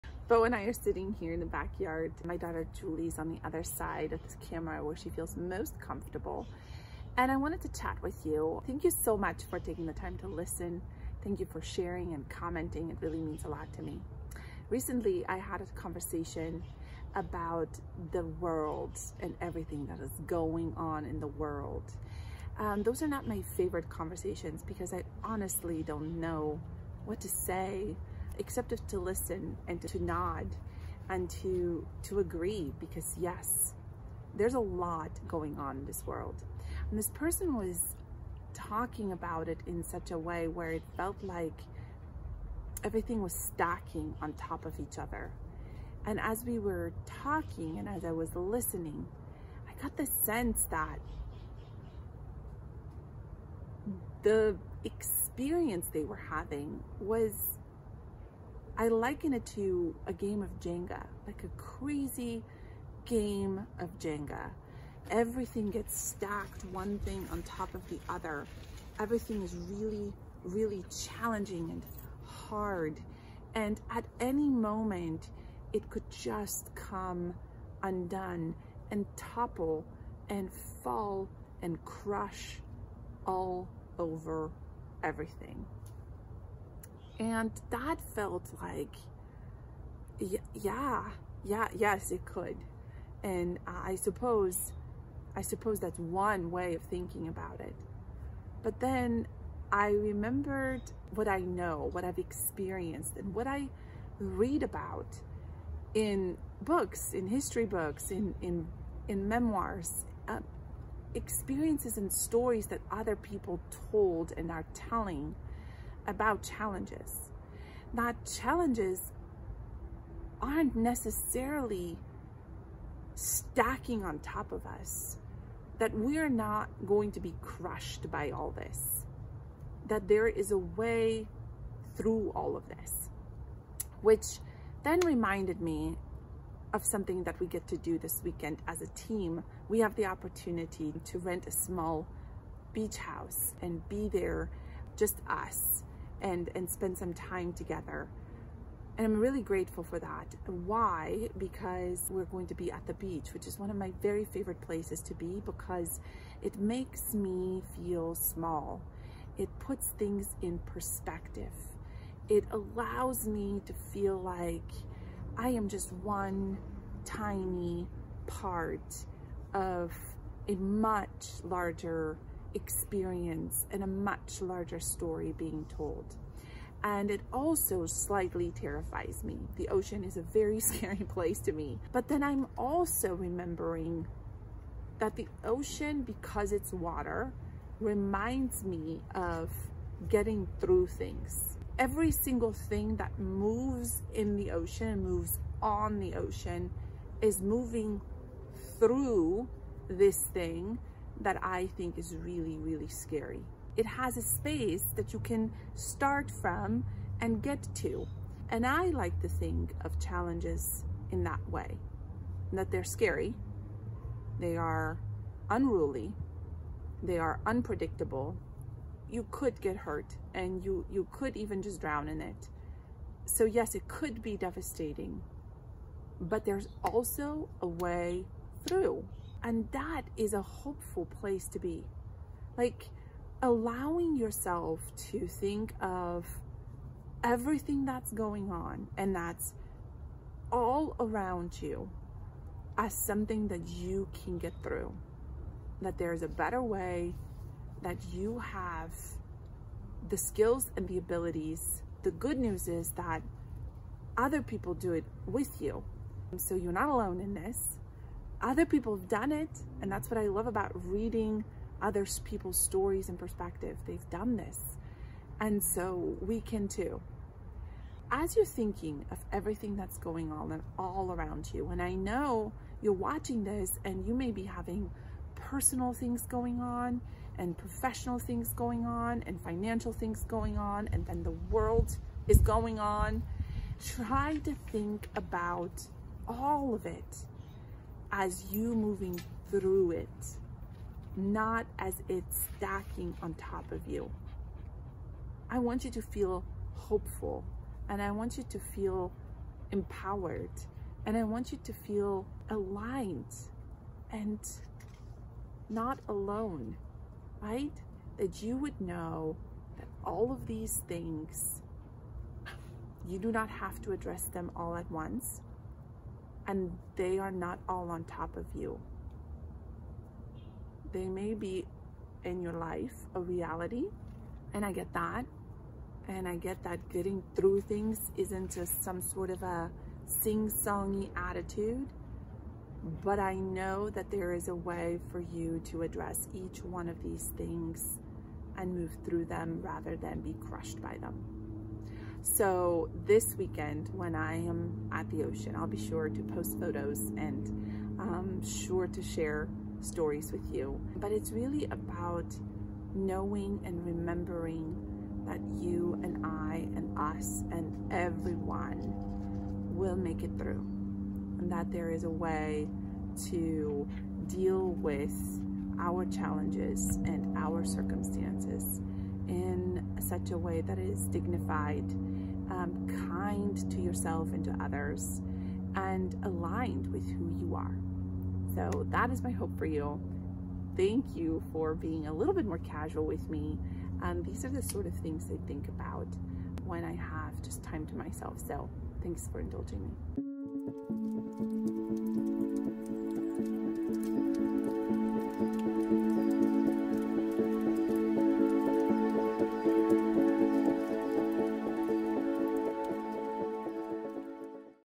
What is going on in the world?! | Bianca Broos
Regaining perspective can be a challenge these unique times.

In this video, I share one way that helps me regain perspective when everything around me feels like I’m navigating stormy seas.